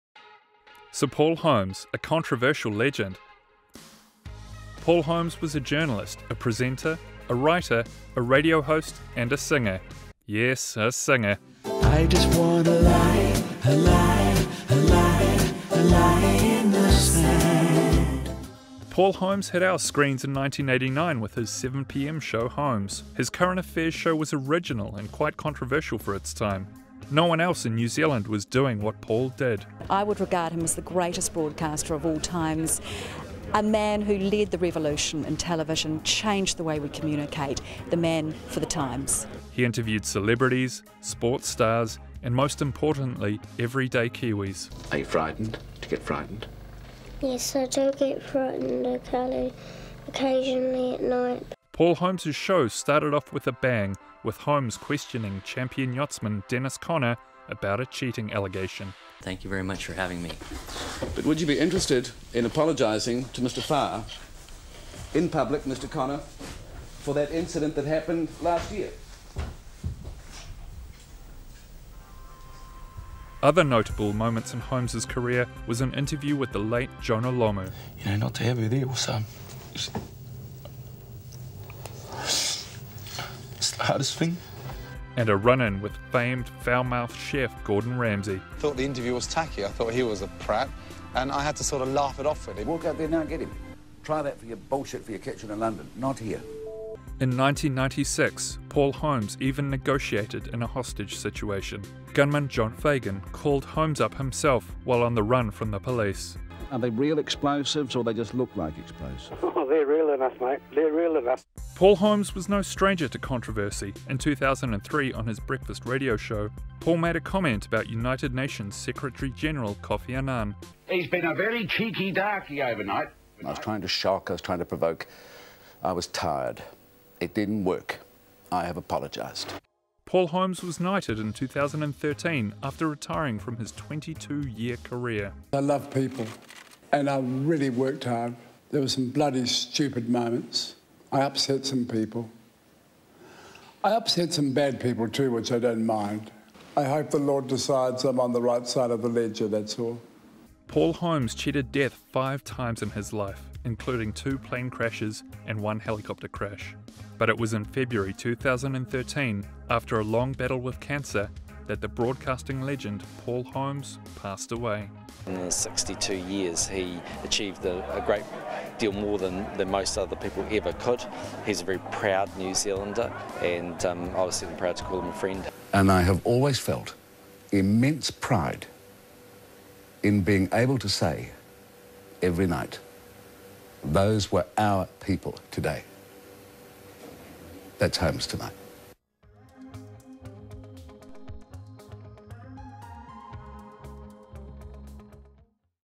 Paul Holmes: a controversial legend
"Those were our people today, and that was Holmes tonight". We re:visit the career of the late Sir Paul Holmes, a broadcasting legend who came with his own brand of controversy.